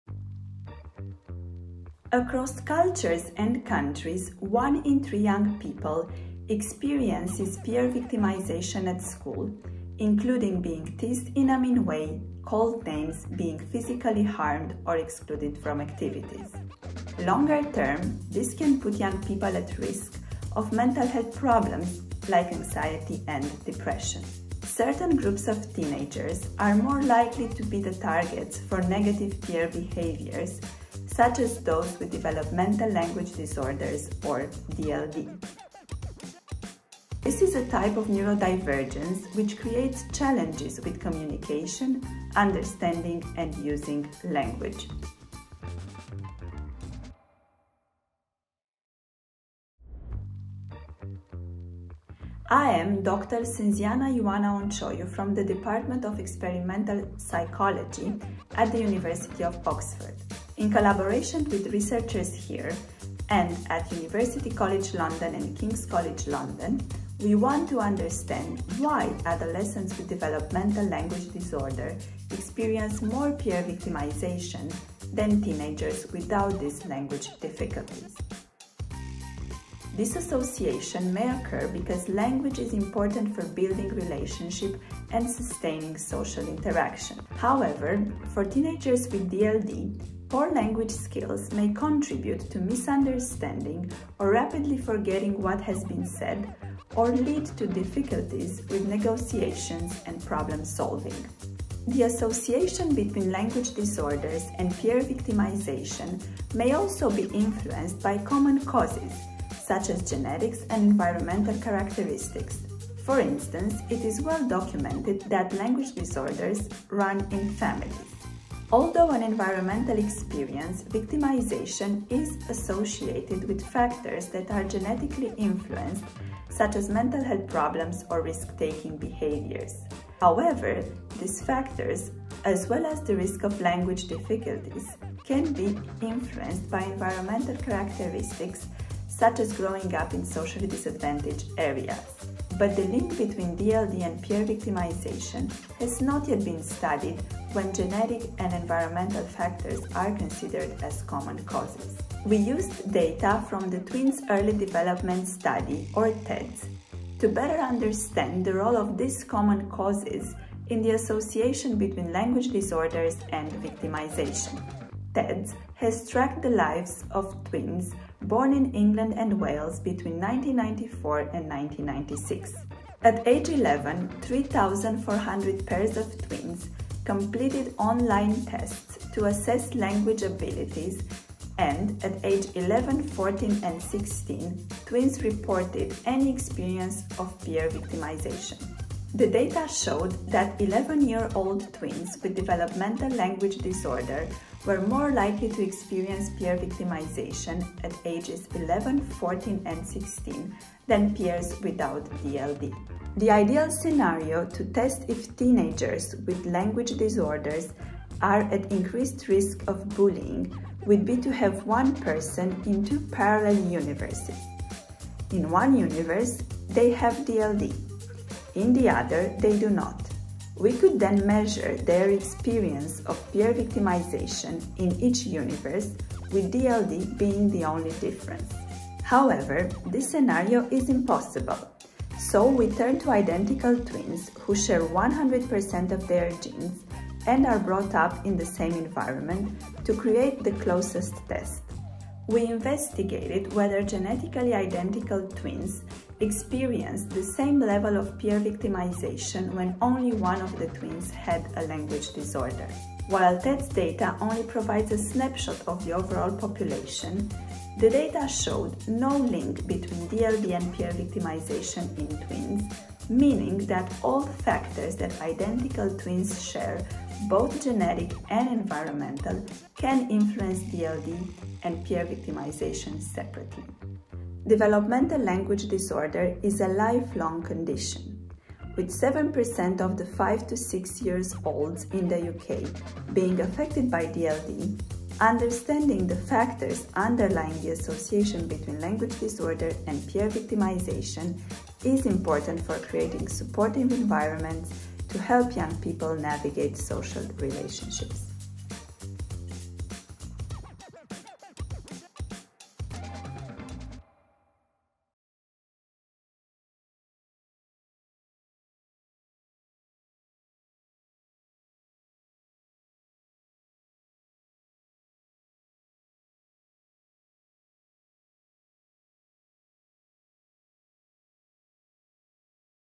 Decoding Developmental Language Disorders and Peer Victimisation in Young People
The best way to determine whether there is a link between Developmental Language Disorders (DLD) and peer victimisation would be to examine one person living in two parallel universes. In one universe they would have DLD and in the other they would not. If they were bullied in one universe and not the other, we might reasonably be able to conclude that DLD was the cause. Of course, this is impossible! So, the next best thing? Identical twins. Dr Sînziana-Ioana Oncioiu from Oxford's Department of Experimental Psychology tells us what she found out from TEDS, one of the world's leading twin studies.